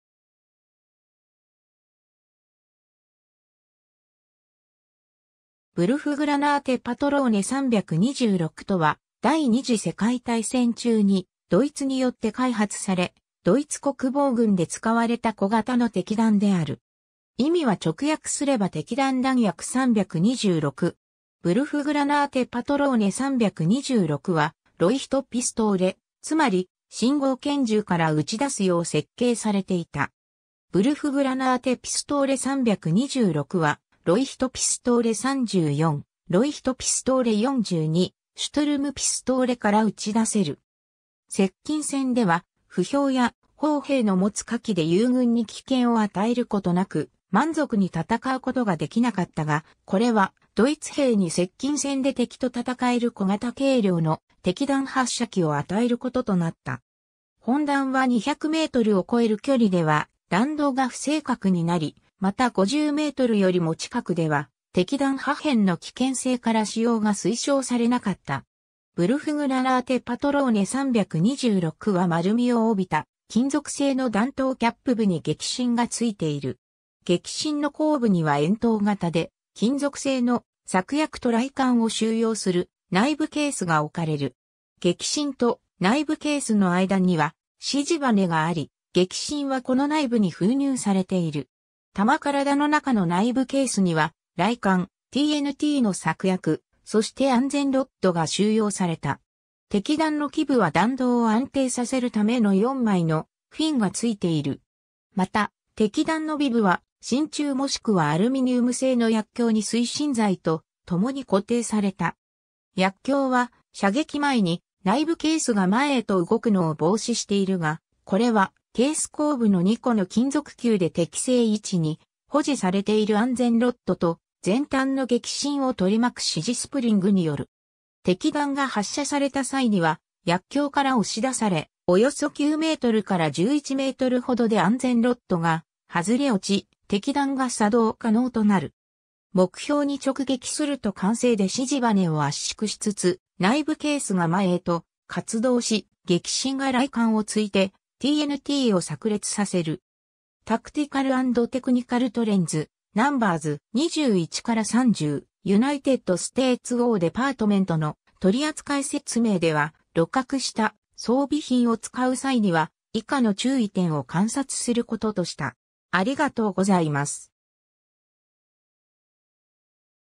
ヴルフグラナーテ・パトローネ326
ヴルフグラナーテ・パトローネ326とは、第二次世界大戦中にドイツによって開発され、ドイツ国防軍で使われた小型の擲弾である。
意味は直訳すれば擲弾弾薬326。
ヴルフグラナーテ・パトローネ326はロイヒトピストーレ、つまり信号拳銃から撃ちだすよう設計されていた。
 ヴルフグラナーテ・ピストーレ326はロイヒトピストーレ34、ロイヒトピストーレ42、シュトゥルムピストーレから撃ち出せる。
接近戦では、歩兵や砲兵の持つ火器で友軍に危険を与えることなく満足に戦う事ができなかったが、これはドイツ兵に接近戦で敵と戦える小型軽量の擲弾発射器を与えることとなった。
本弾は200mを越える距離では弾道が不正確になり、また50mよりも近くでは擲弾破片の危険性から使用が推奨されなかった。
 ヴルフグラナーテ・パトローネ326は丸みを帯びた金属製の弾頭キャップ部に撃針が付いている。
撃針の後部には円筒形で金属製の、炸薬と雷管を収容する内部ケースが置かれる。
撃針と内部ケースの間には支持バネがあり、撃針はこの内部に封入されている。
弾体の中の内部ケースには雷管、TNTの炸薬、そして安全ロッドが収容された。
擲弾の基部は弾道を安定させるための4枚のフィンが付いている。
また擲弾の尾部は、真鍮もしくはアルミニウム製の薬莢に推進剤と共に固定された。
薬莢は射撃前に内部ケースが前へと動くのを防止しているが、これはケース後部の2個の金属球で適正位置に保持されている安全ロッドと、前端の撃針を取り巻く支持スプリングによる。
擲弾が発射された際には薬莢から押し出され、およそ9mから11mほどで安全ロッドが外れ落ち、擲弾が作動可能となる。
目標に直撃すると慣性で支持バネを圧縮しつつ内部ケースが前へと滑動し、撃針が雷管を突いてTNTを炸裂させる。
 『Tactical And Technical Trends, Nos. 21-30. United States War Department』の取り扱い説明では、鹵獲した装備品を使う際には以下の注意点を観察することとした。